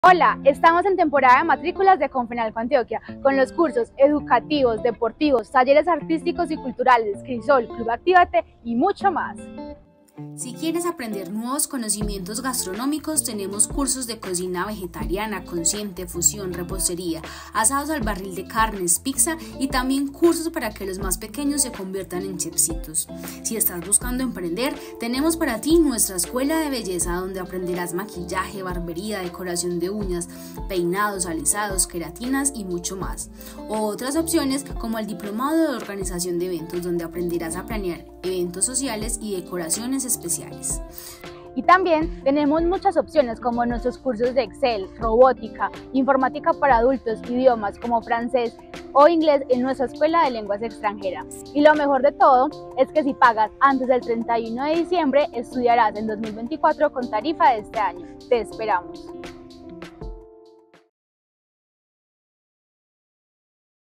Temporada de matrículas
¡Despierta tu potencial en Comfenalco Antioquia! 🚀 Estamos en temporada de matrículas, ofreciéndote un abanico de oportunidades educativas, deportivas, talleres artísticos y culturales, así como experiencias en nuestro programa Crisol, Club Actívate y ¡mucho más!

¡No te pierdas esta oportunidad y conecta con nuevos conocimientos! Si realizas tu pago antes del 31 de diciembre, tendrás la oportunidad de estudiar en enero de 2024 con tarifas del 2023.

¡El futuro te espera con las puertas abiertas en Comfenalco Antioquia! 🌟 ¡Inscríbete en el siguiente link y haz de tus sueños una realidad! 📚💡